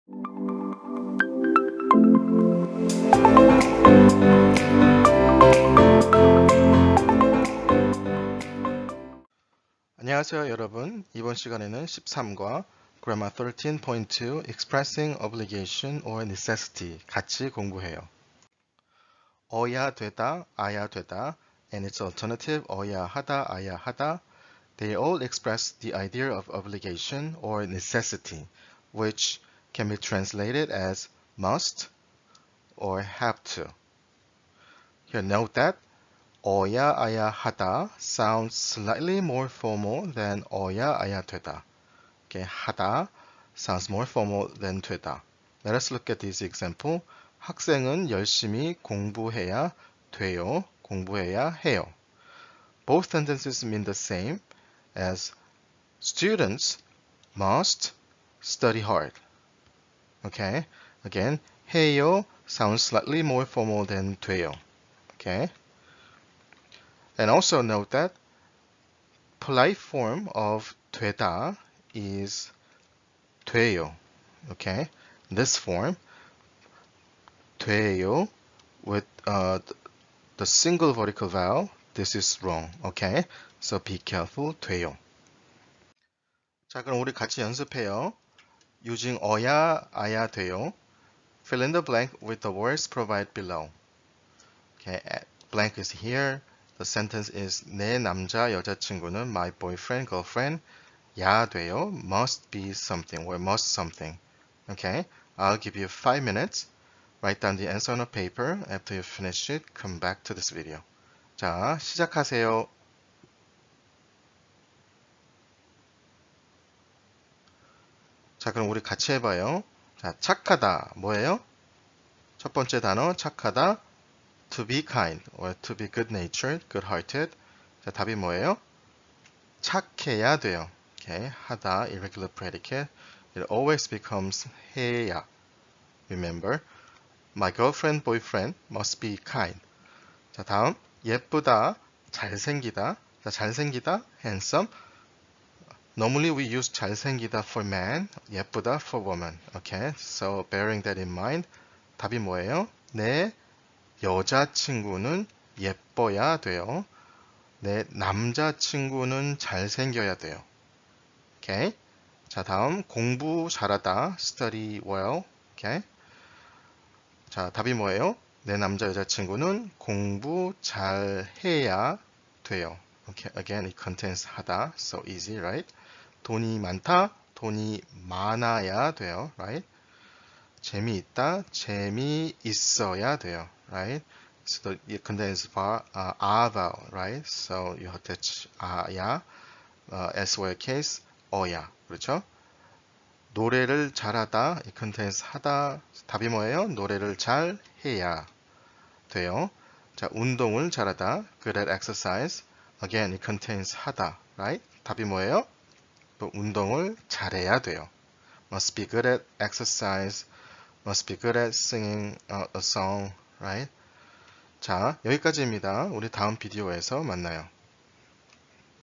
QC KOR102 L13C1 G13.2
Lesson 13 Conversation 2
G13.2 Expressing obligation or necessity ~어/아야 되다/하다